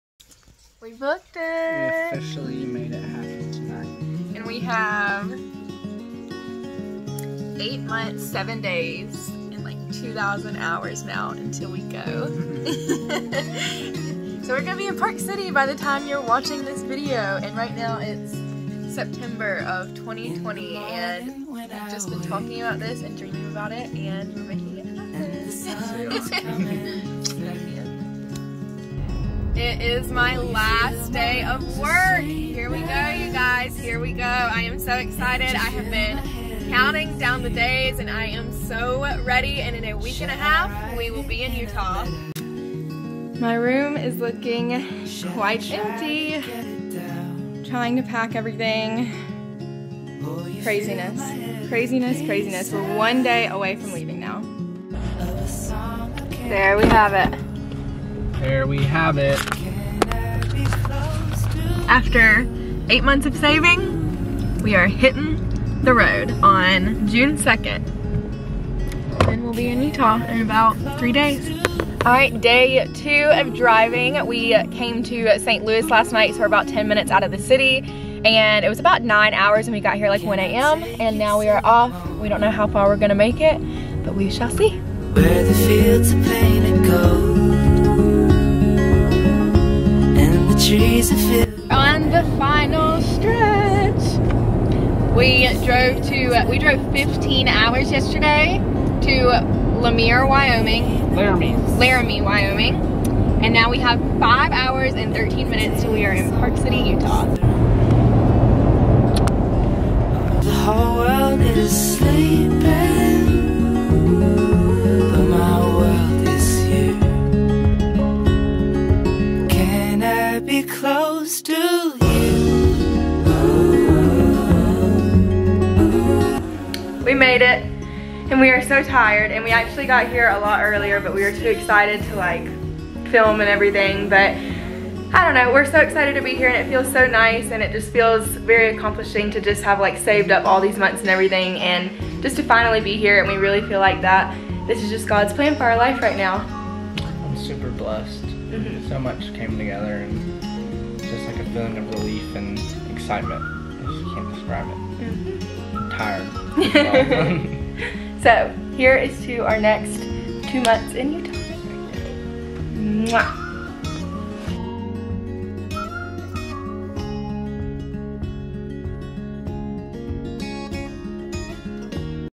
The Road to Utah | Our Summer Adventure
Just a short recap of our trip coming out to Utah! It's been 8 months in the making so get ready for some exciting videos to come!

XOXO